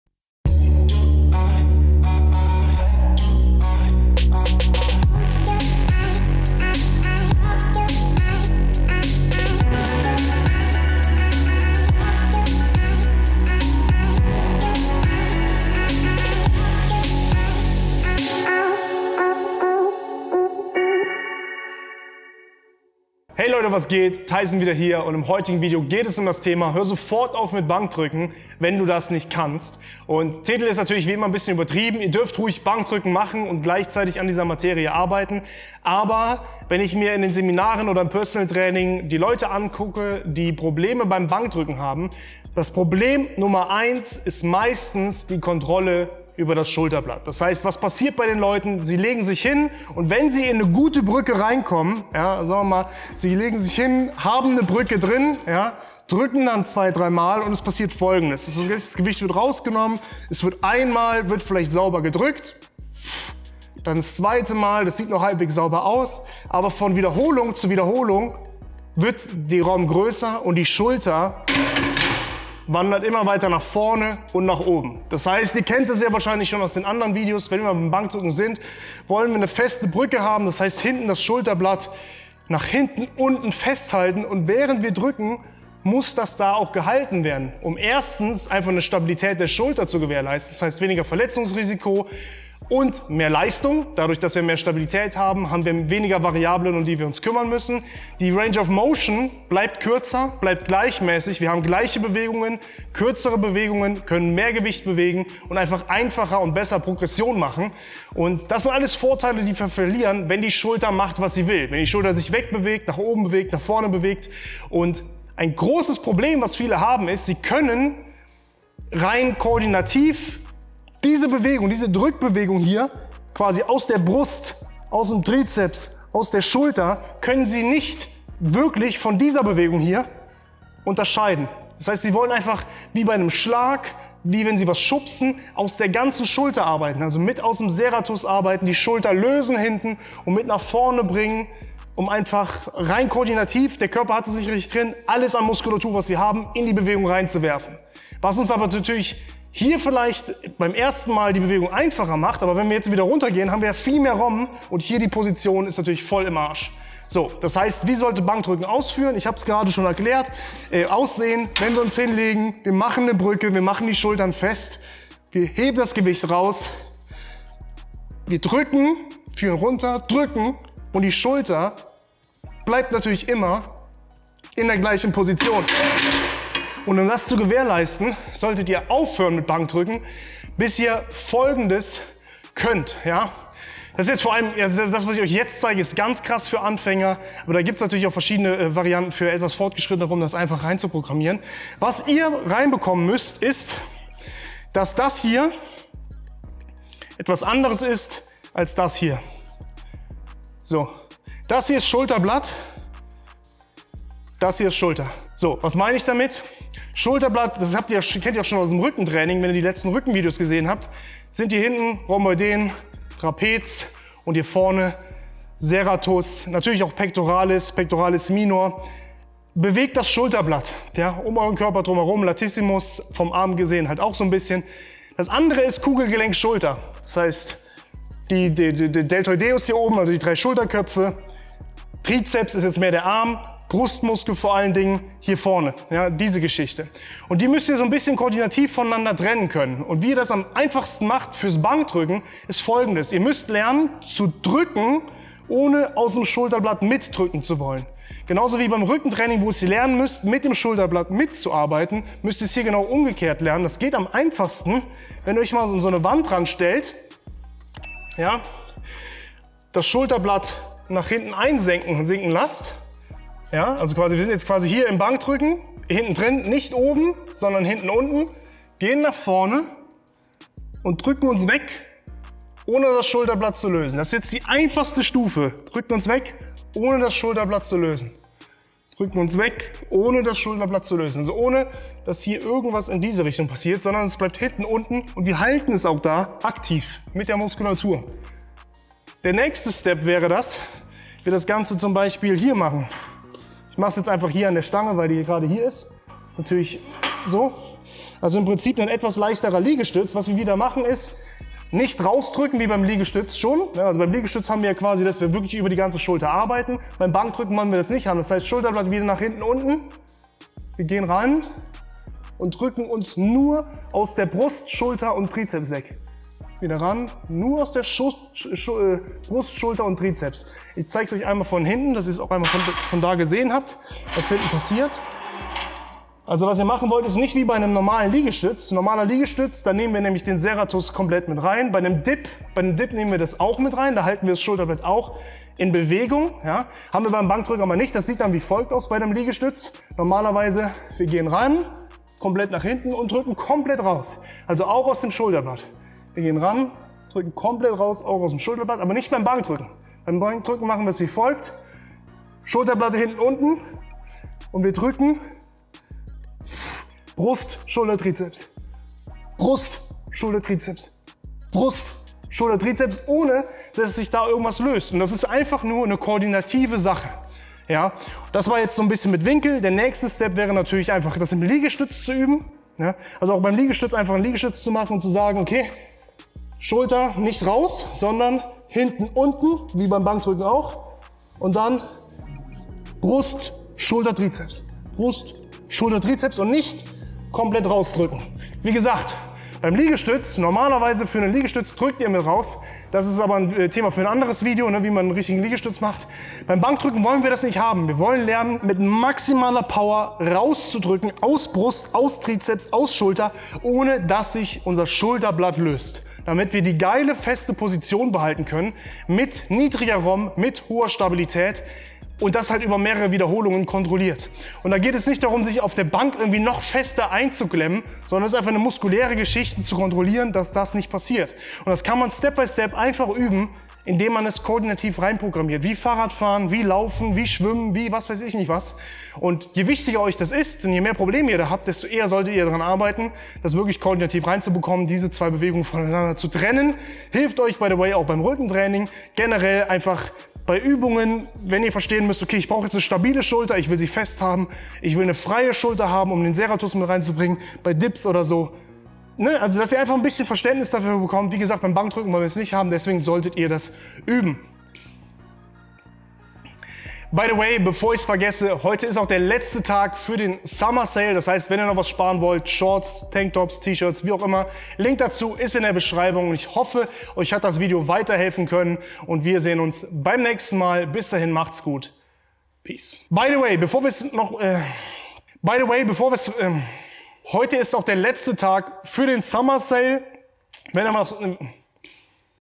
Bankdrücken SOFORT verbessern! Das MUSST du können!
In diesem Video zeige ich euch den wichtigsten Part beim Bankdrücken, den leider nur die wenigsten wirklich drauf haben!